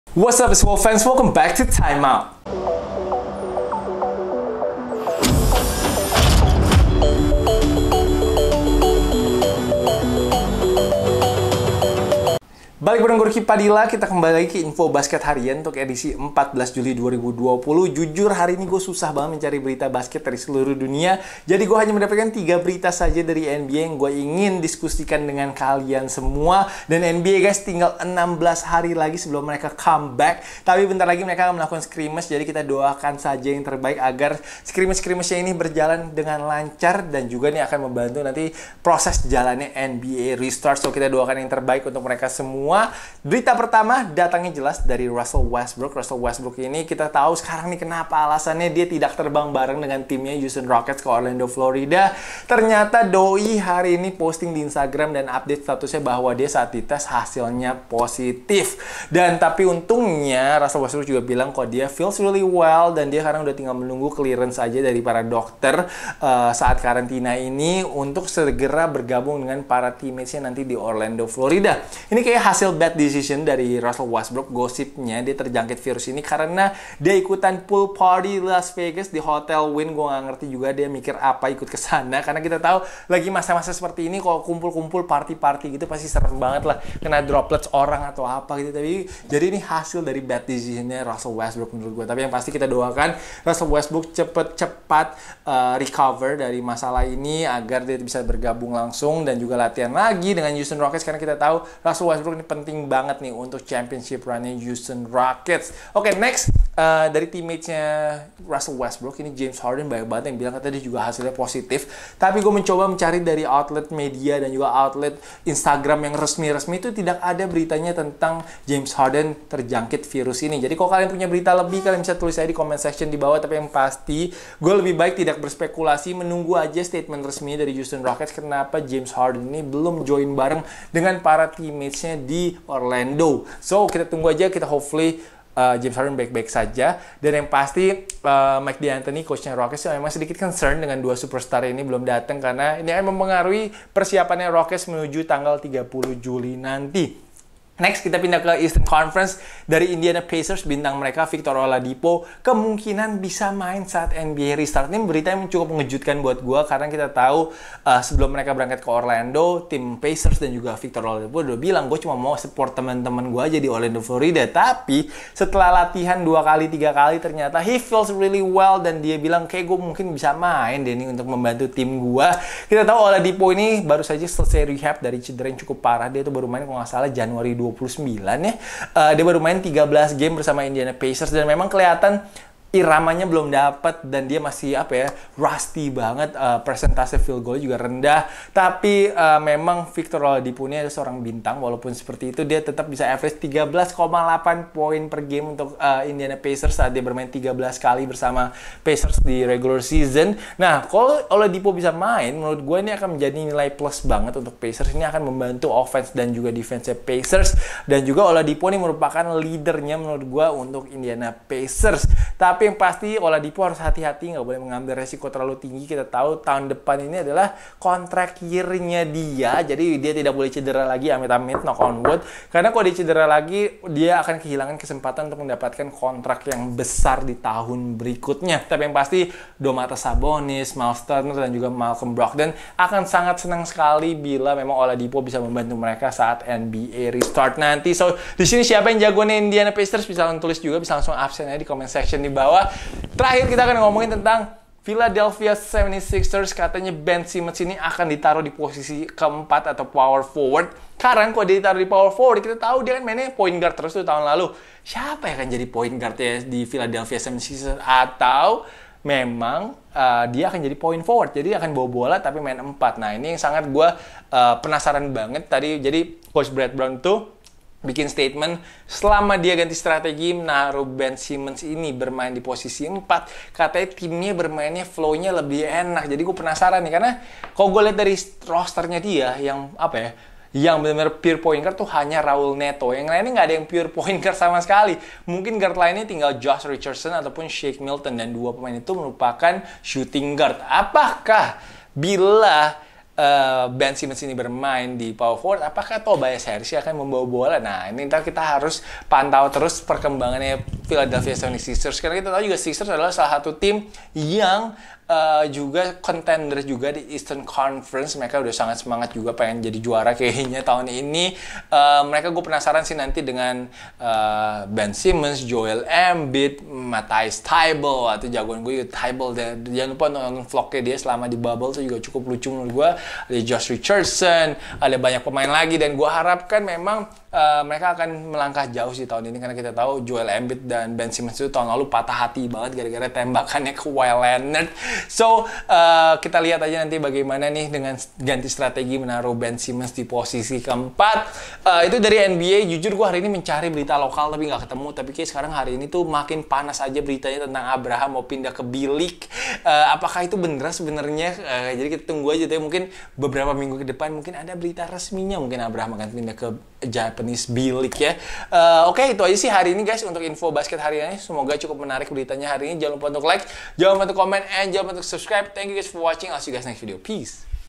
Time Out 14 Juli 2020: Victor Oladipo Mungkin Main // Ben Simmons Akan Main Power Forward
Info basket harian 14 Juli 2020.

Thumbnail photo via @Vicoladipo